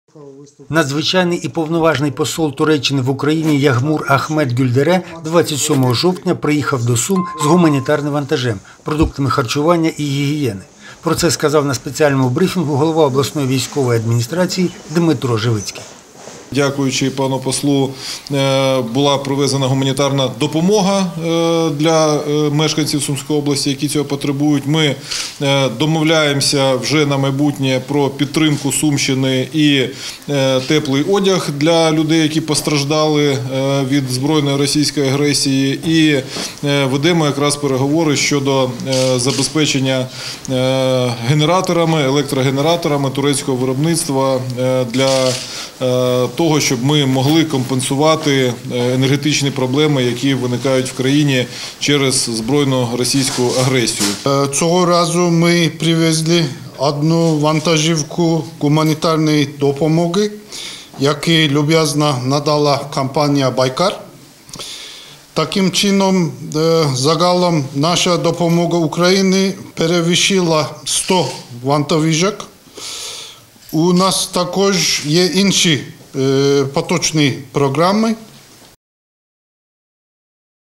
Турецький посол привіз у Суми гуманітарний вантаж від компанії “Байкар”
Надзвичайний і Повноважний Посол Туреччини в Україні Ягмур Ахмет Гюльдере 27 жовтня приїхав до Сум з гуманітарним вантажем: продуктами харчування та гігієни.